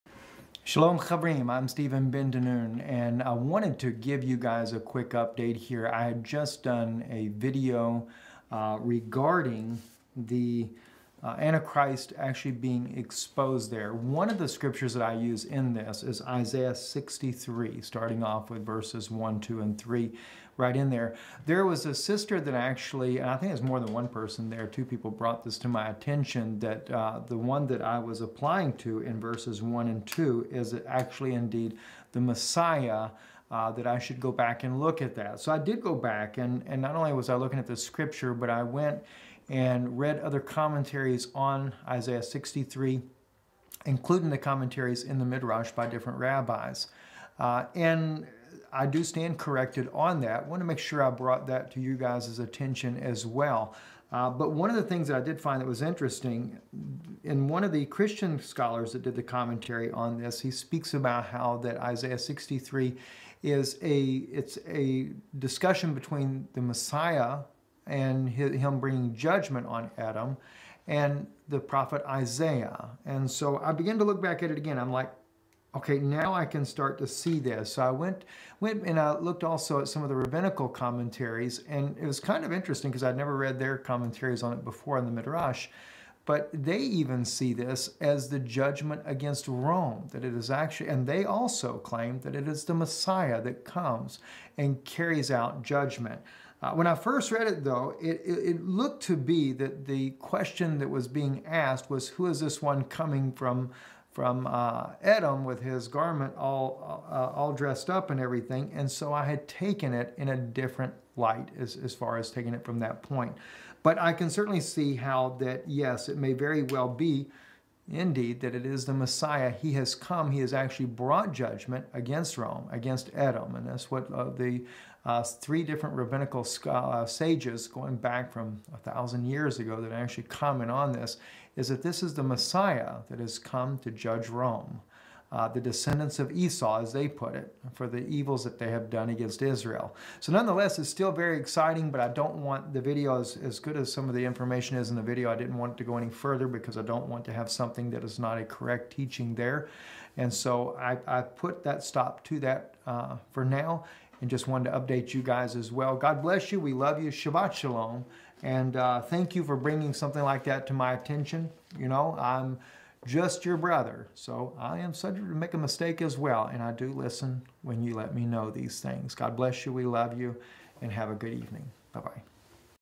Correction on last video
A dear sister brought up a mistake I had made in quoting Isaiah 63 and after additional research I must agree with the sisters correction. I never desire to mislead anyone so I will remove the video. and then redo this teaching. In fact as I saw the Rabbinical commentaries on Isaiah 63 it only shows a deeper connection of the Judgment of Rome. Thank you my sister for bringing this to my attention so that I may be Faithful to Christ Yeshua and His People